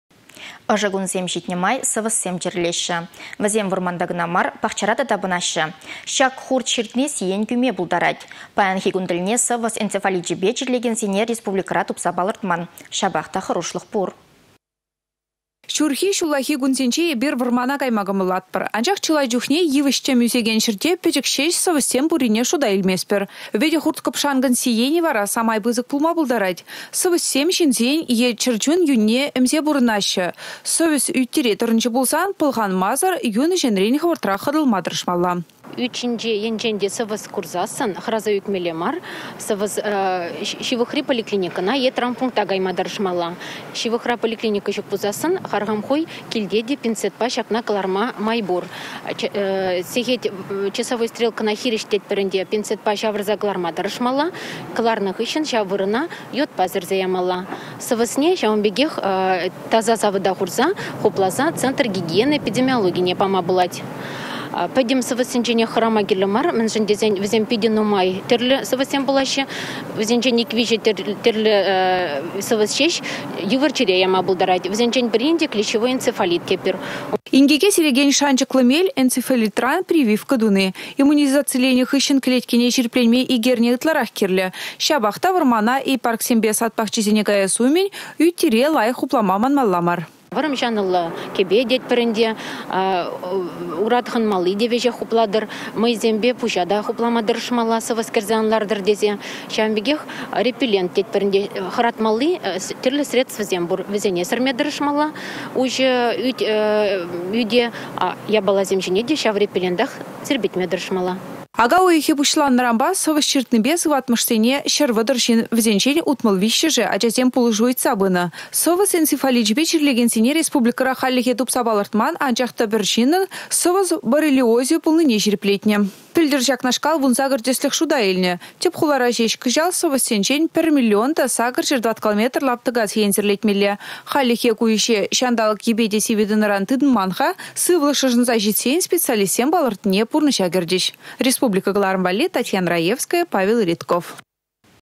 Сăвăсран мĕнле сыхланмалла?
Ăшă   кунсем çитнĕ май сăвăссем чĕрĕлеççĕ. Вĕсем  вăрманта кăна мар, пахчара та тапăнаççĕ. Çак хурт çыртни сиен кӳме пултарать. Паянхи кун тĕлне сăвăс энцефаличĕпе чирлекенсене республикăра  тупса палăртман.  Çапах та хăрушлăх пур.